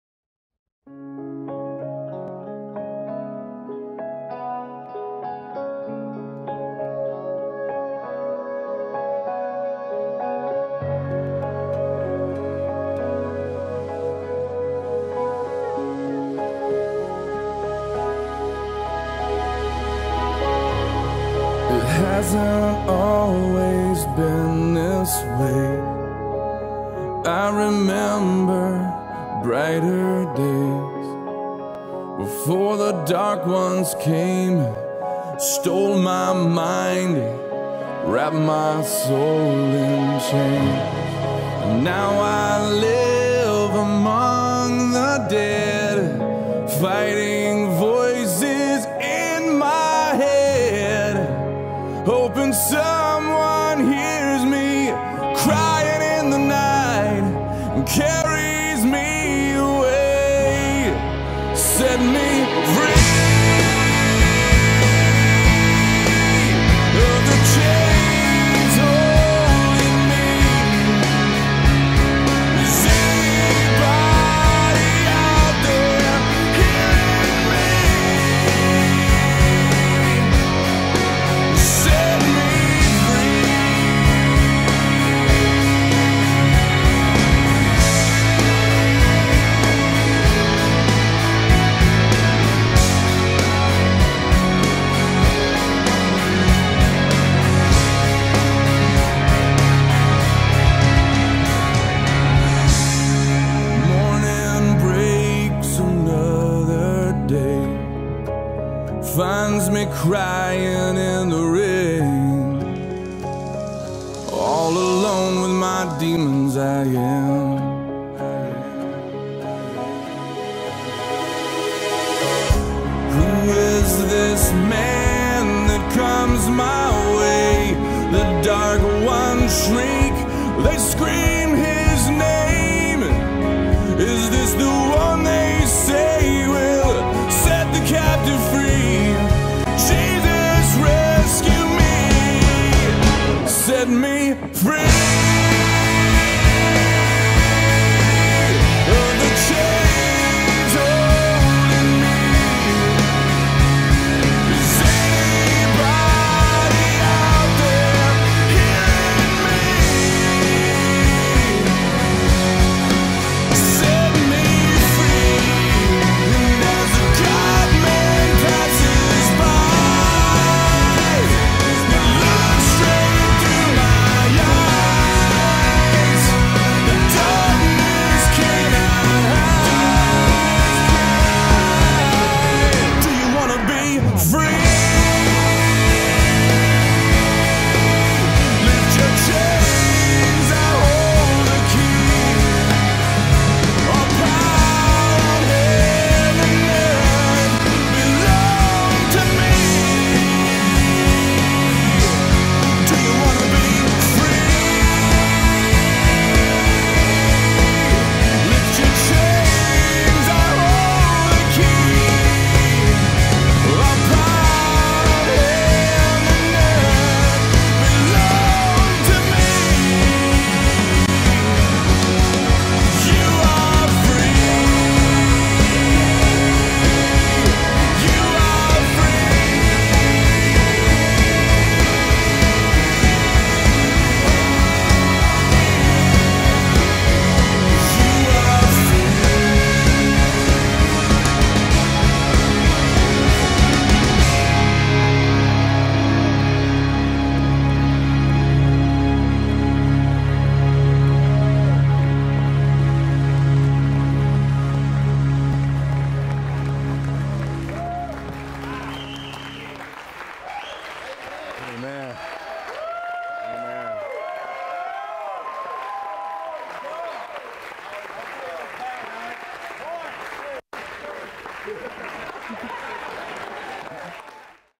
Celebrate Recovery - True Purpose - Set Me Free Skit
True Purpose Ministries blessed Celebrate Recovery at RIO Revolution with this amazing, powerful Set Me Free Skit.

This video is property of Celebrate Recovery at RIO Revolution, 3419 East Lamar Alexander Pkwy, Maryville, TN 37804.

True Purpose Ministries is located in Maryville, TN. Thank you guys for all your had work to perform this masterpiece!!